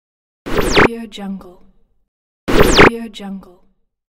Sound - Futuristic Glitch | AudioJungle Download
Download Sound - Futuristic Glitch | AudioJungle

Category : Futuristic Sounds. Sound

Electronic glitch sound of short futuristic event. Digital sound effect for app. game or animation.